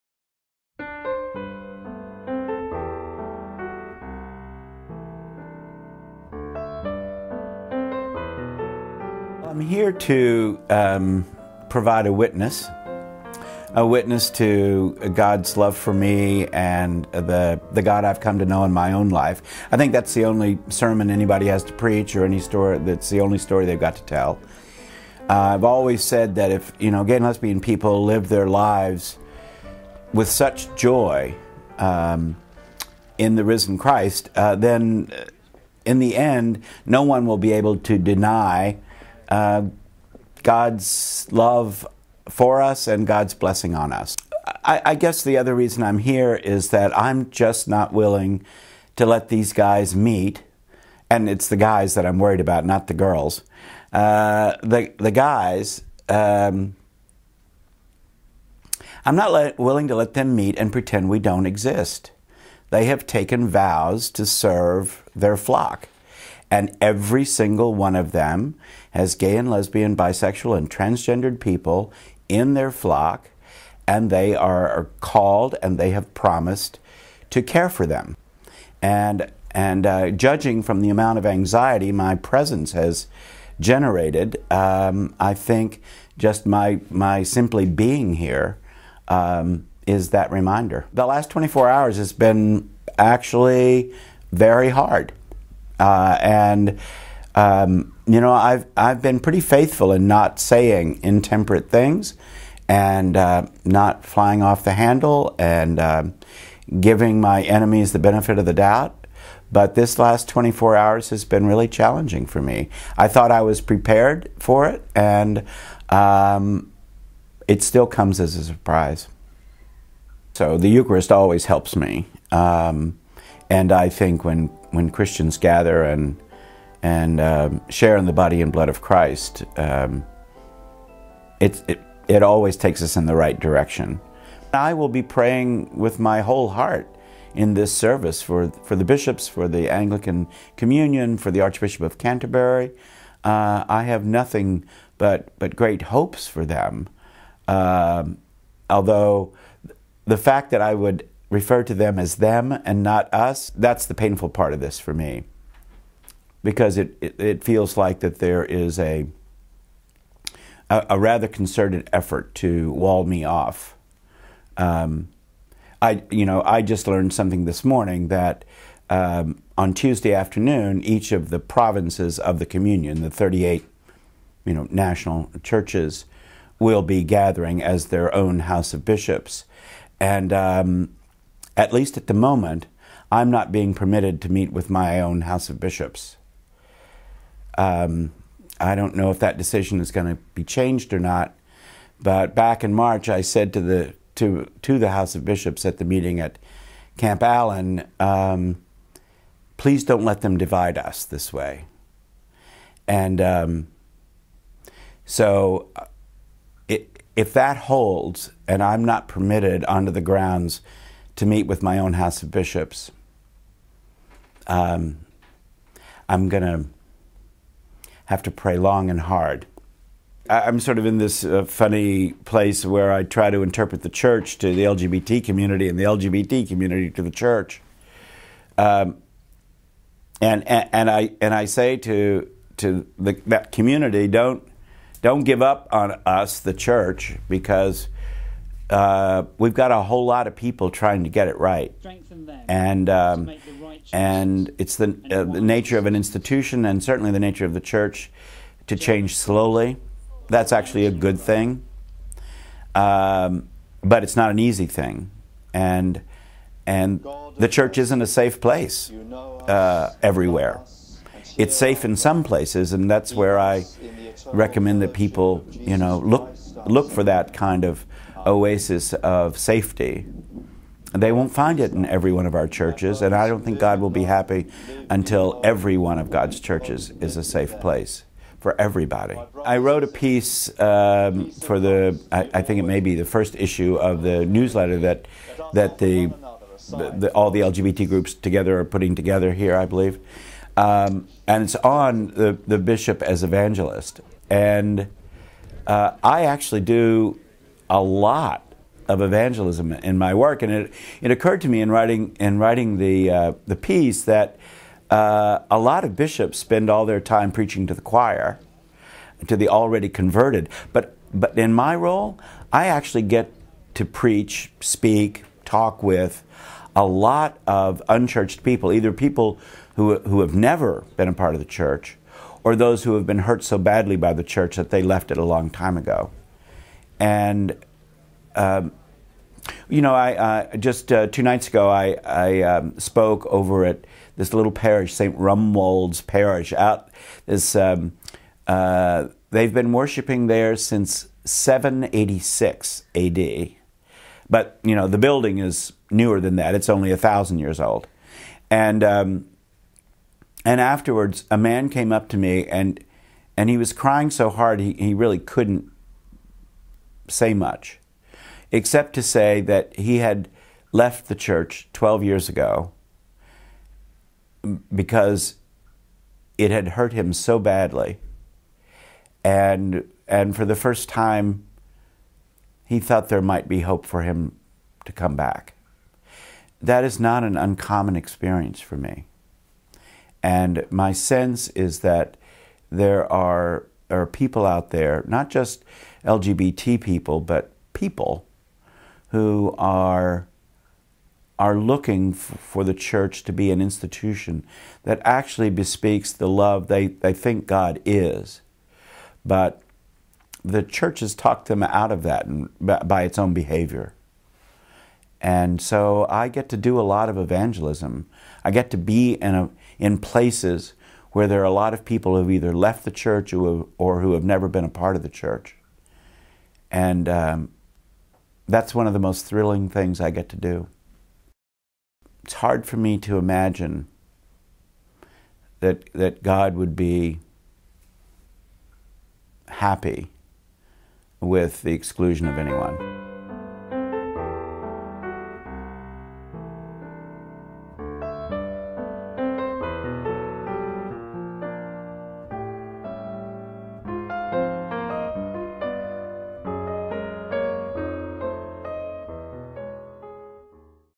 Gene prior to the Integrity/ Changing Attitude Eucharist
An interview with the bishop of New Hampsire, the Rt. Rev. V Gene Robinson, prior to the Integrity/ Changing Attitude Eucharist on July 20, 2008.